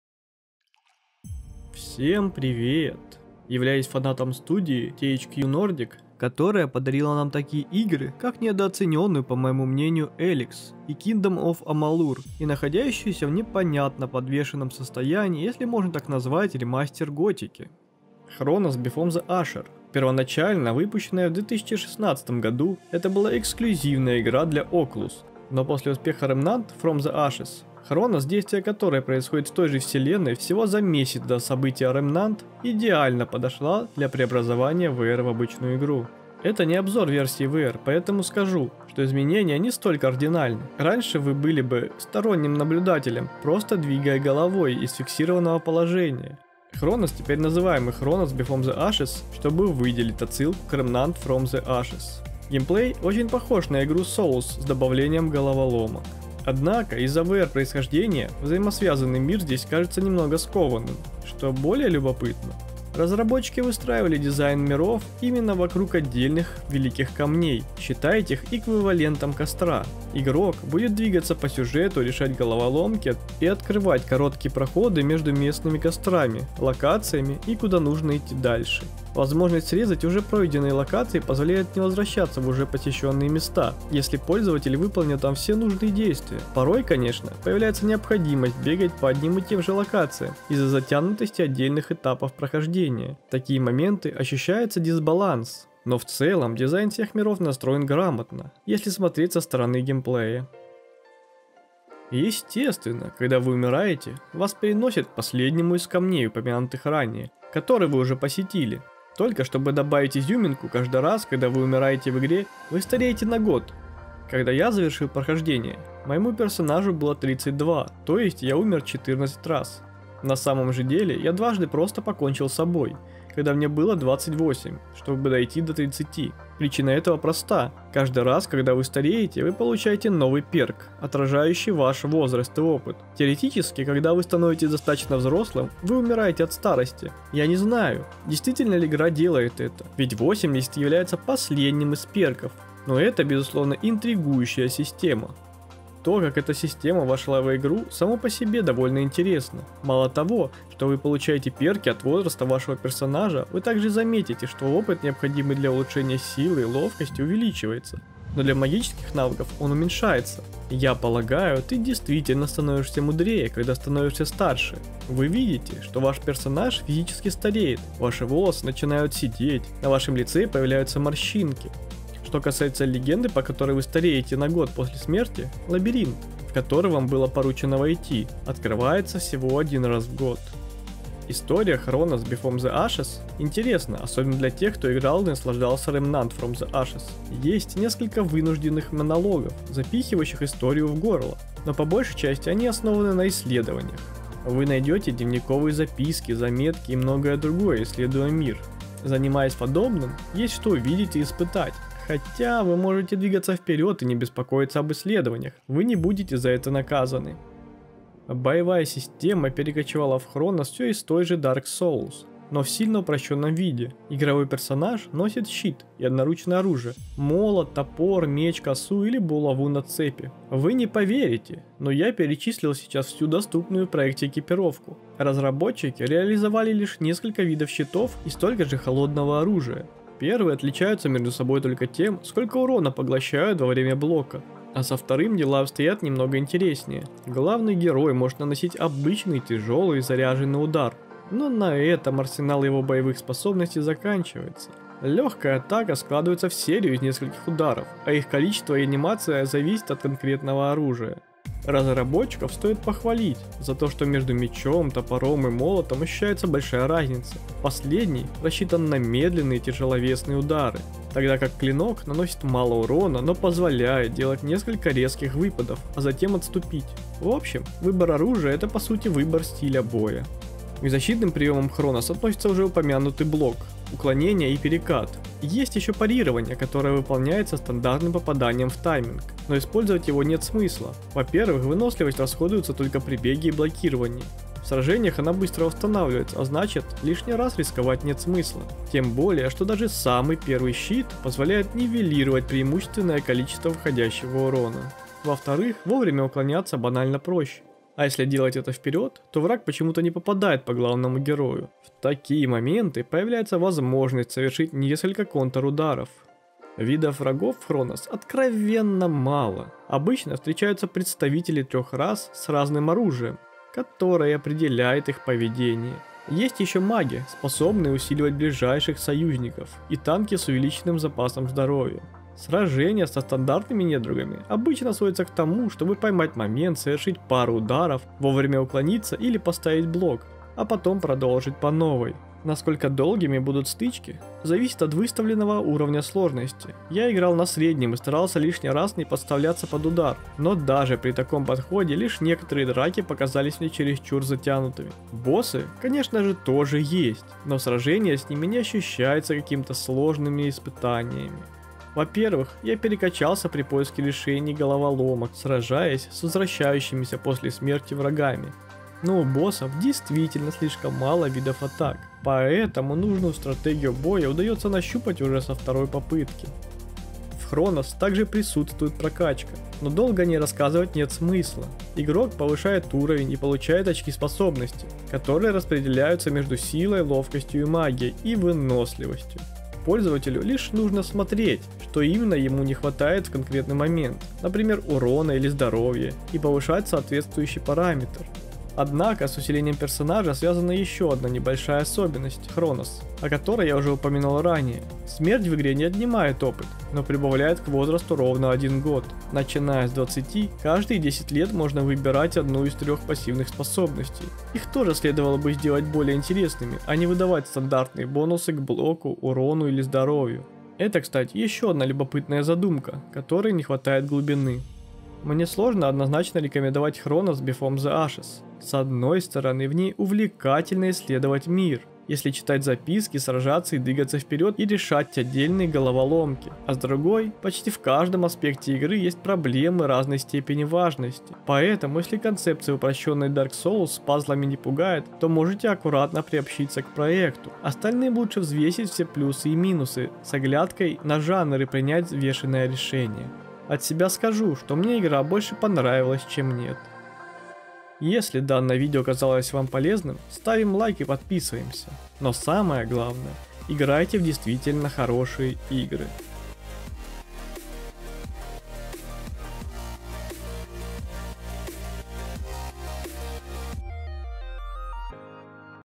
Что не так с игрой - Chronos: Before the Ashes | Обзор
Мое почтение! В этом видео вы узнаете о новой игре студии THQ Nordic - Chronos: Before the Ashes,  почему у нее такое знакомое название?